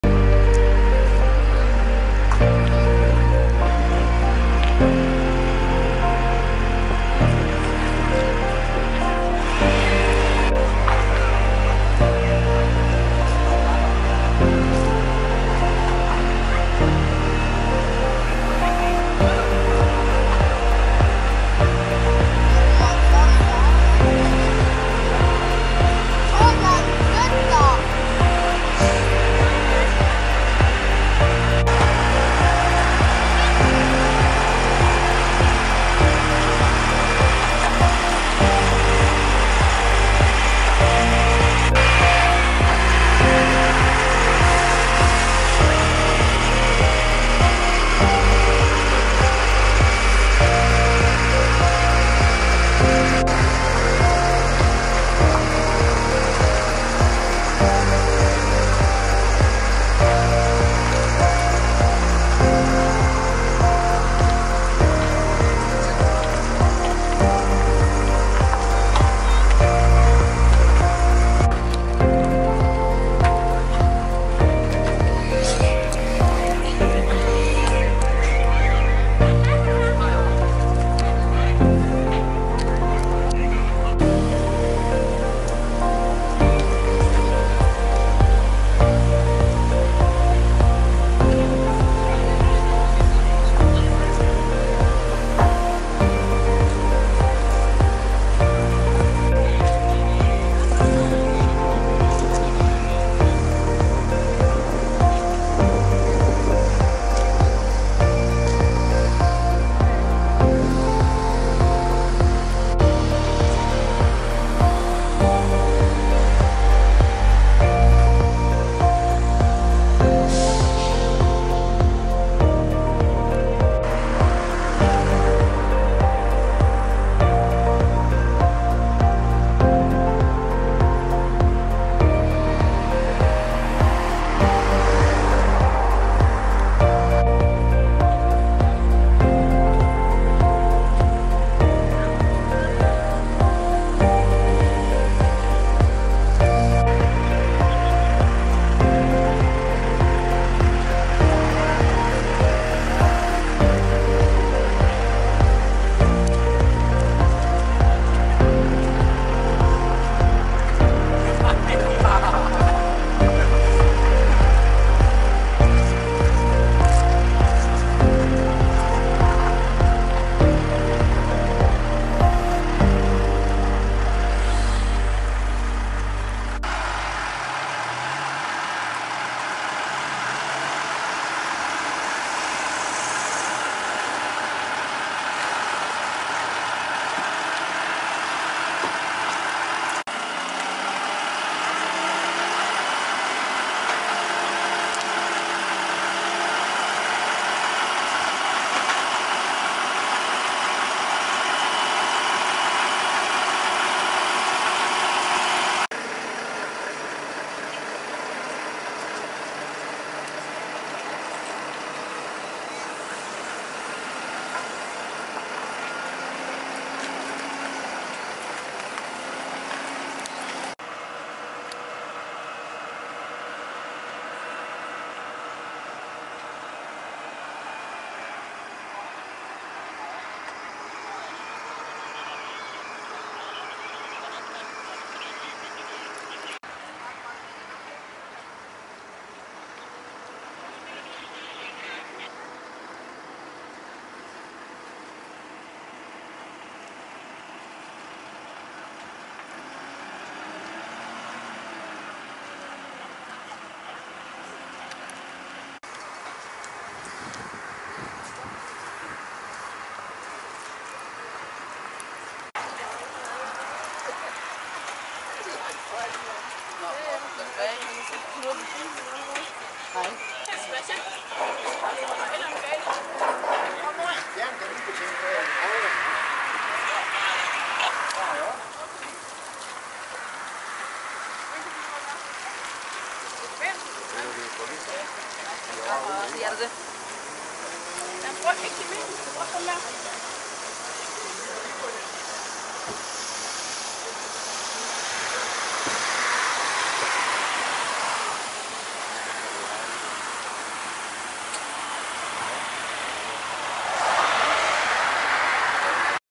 Volvo truck transporting huge 150 ton load in Denmark
Transporting huge 83x6 m and 150 ton load in Denmark